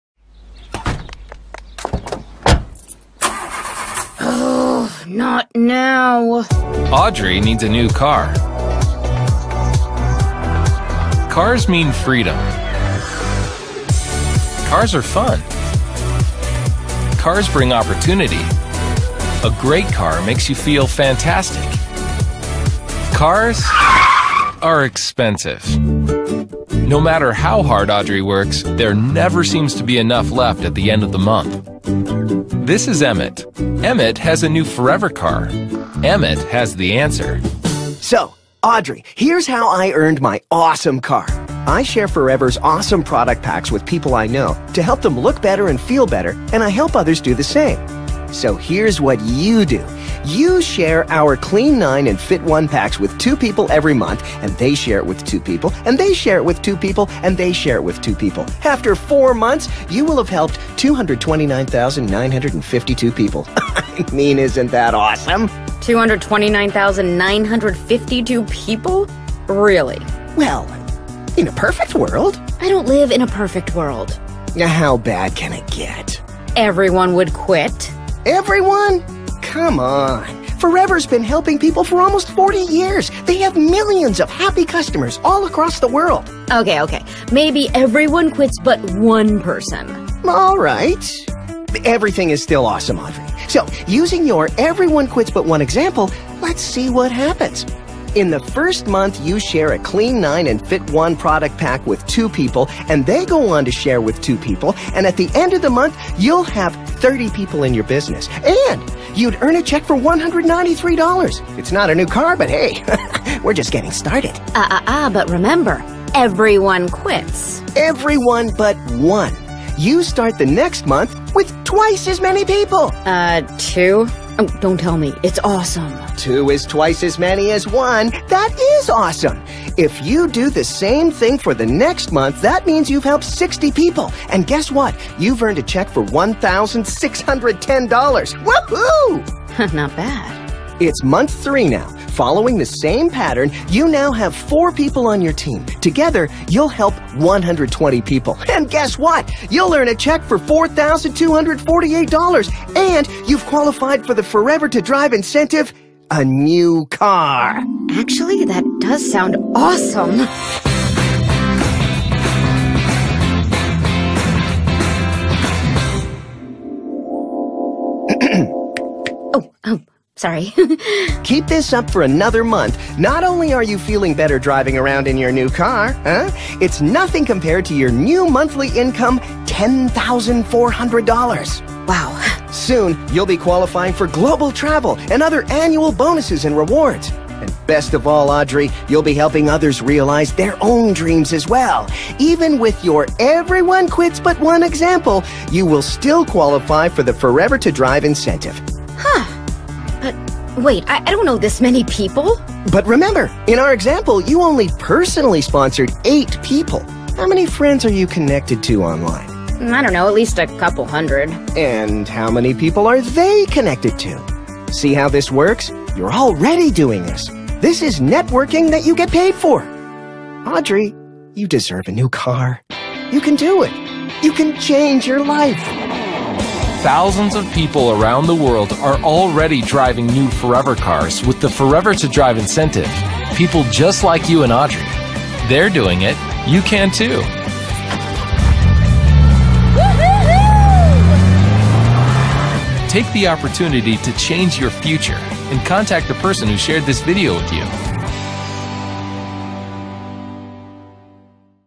FOREVER 2 DRIVE Drive a Car Paid By Forever Living
Would  you like a new car?
Check out this clip to see how you could get one by simply introducing a hand full of people to some wonderful products.